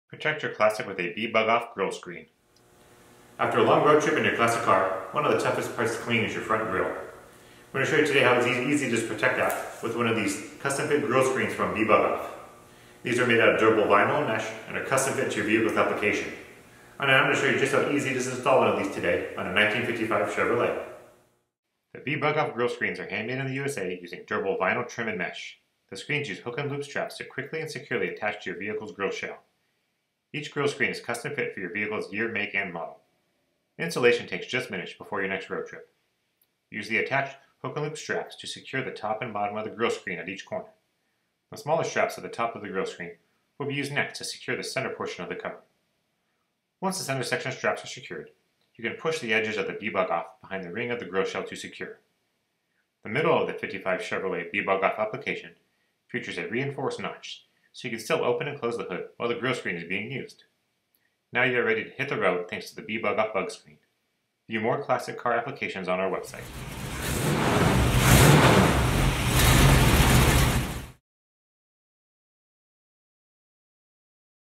Bbugoff Custom Fit Classic Car Grill Mesh Bug Screen at California Car Cover
On your next road trip install a BBUGOFF screen to protect the grill, radiator, and engine bay from bugs, rocks, and road debris. Custom-fit for your classic car, the bug screen offers superior quality, fit, and finish.

Our video shows you just how easy it is to install a BBUGOFF mesh grille screen on a 1955 Chevrolet. The video also shows the detail and quality on each screen.

Use a dry cloth or brush to easily remove bugs, and mild soap and water when needed for clean-up – without damaging the bug screen. Installs securely with hook and loop straps. Handmade in the USA.